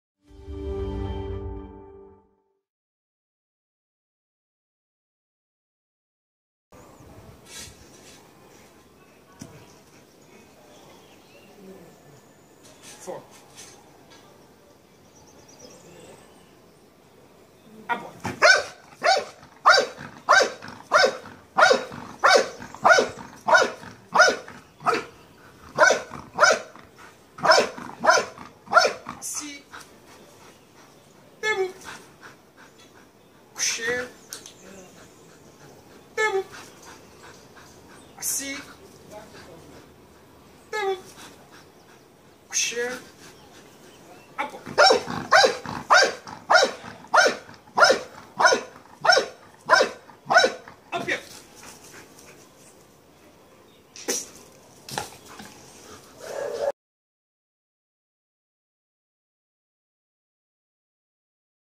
VARIAÇÕES COM BOLA | Piloto Canil Evolution Dogs Training
O vídeo apresenta a cadela Éva da Raça Pastor Belga Malinois no exercício de variações com bola, demonstrando concentração, alegria e vontade em executar os comandos. Para realizar este tipo de atividade, procure sempre o auxilio de um profissional.